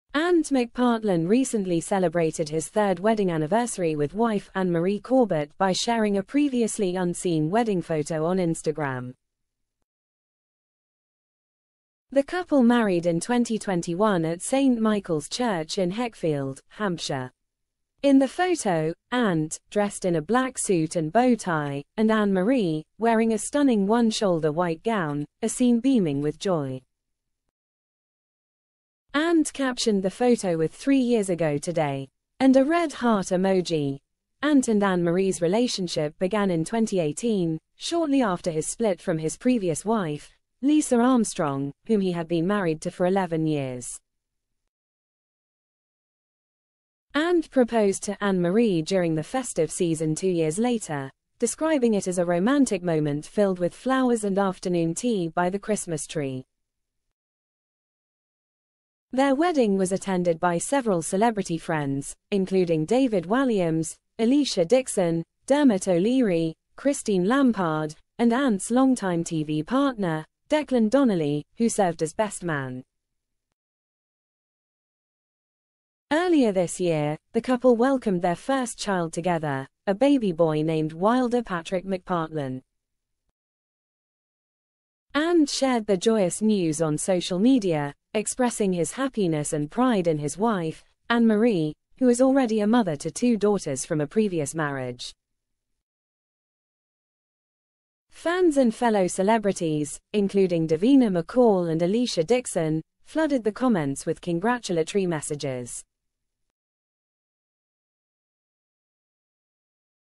Ant McPartlin marks third anniversary with wife Anne Marie with unseen wedding photo
Ant McPartlin is celebrating his third wedding anniversary with wife Anne-Marie Corbett and took to Instagram to share a previously unseen photo from their big day in honour of the milestone occasion#UKCELEBRYTI#CELEBRYTI#UKSHOWBIZ#Showbiz#AntMcPartlin#PhoebeTonks#AnneMarie#ITV